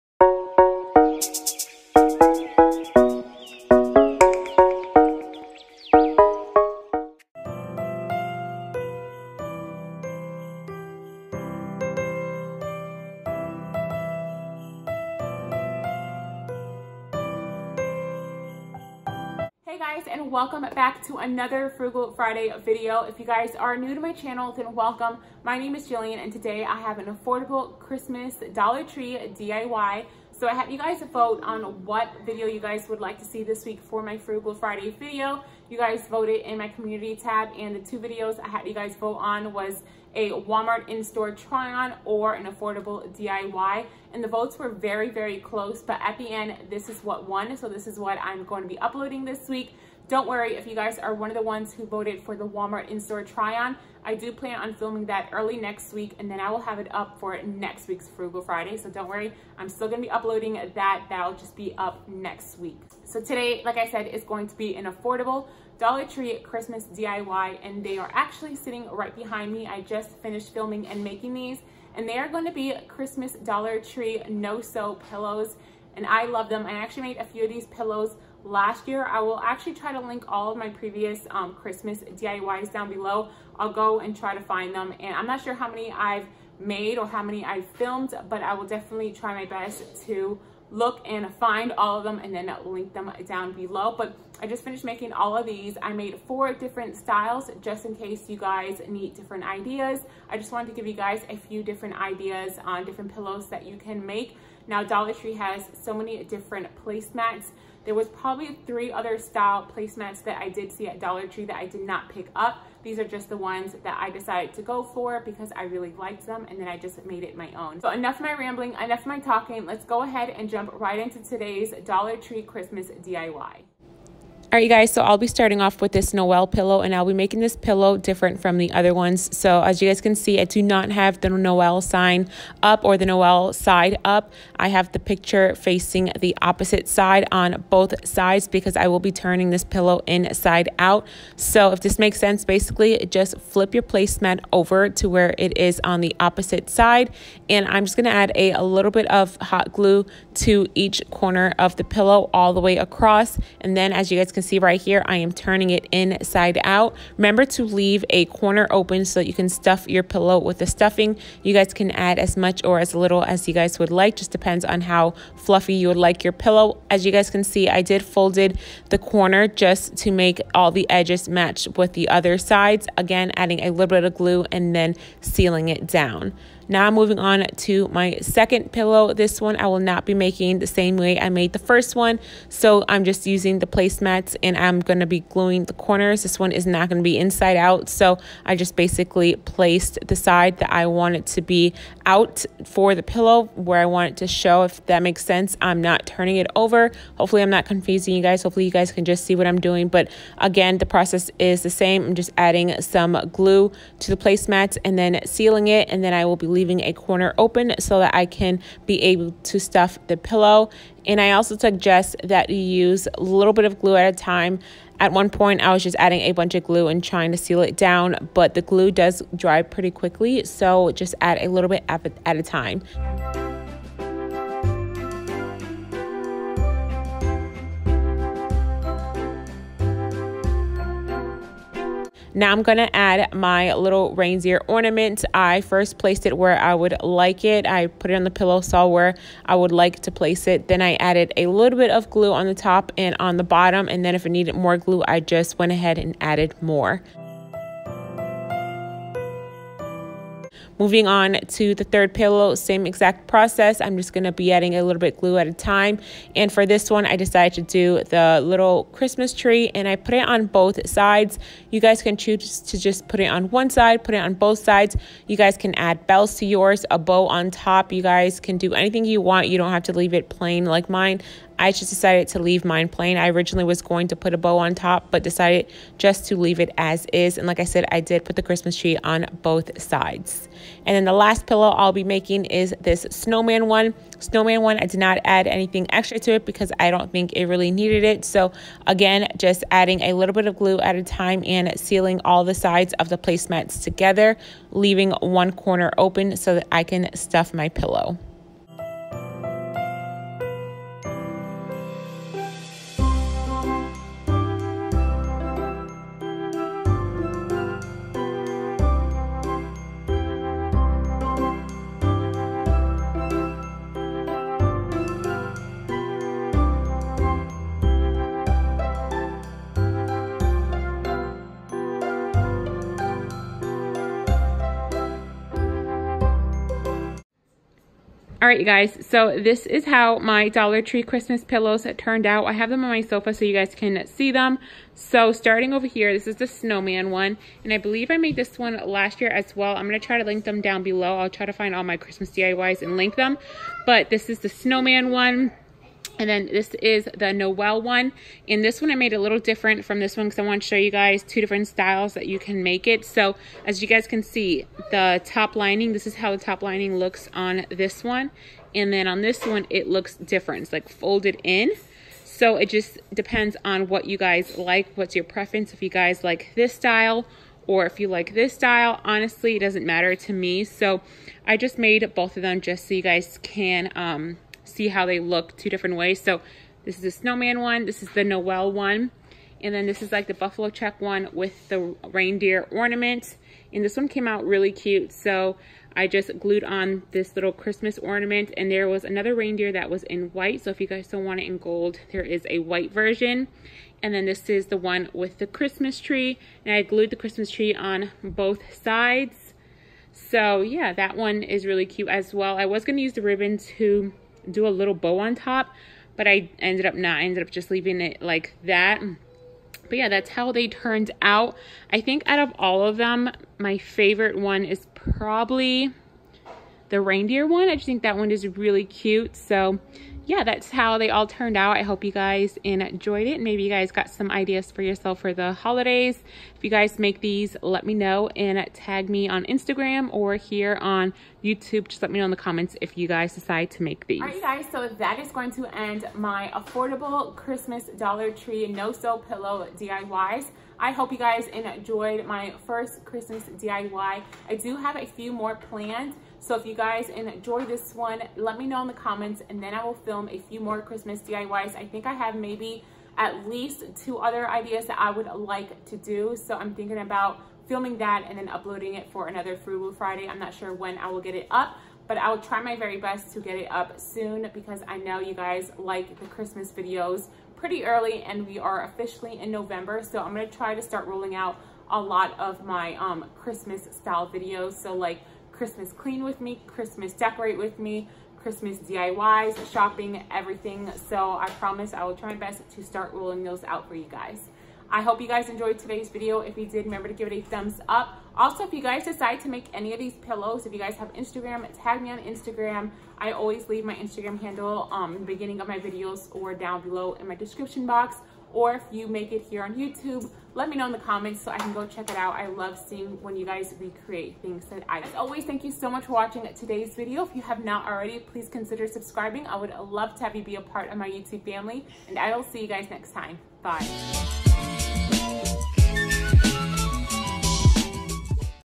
DOLLAR TREE CHRISTMAS DIY | CHRISTMAS DIY PILLOWS | FRUGAL FRIDAY
Hey friends! You guys voted for an affordable diy, for this weeks Frugal Friday. So I decided to make a Christmas diy, hope you liked these affordable pillows.

*PREVIOUS CHRISTMAS DIYS*

JillianS502

Filming Equipment

Music Credit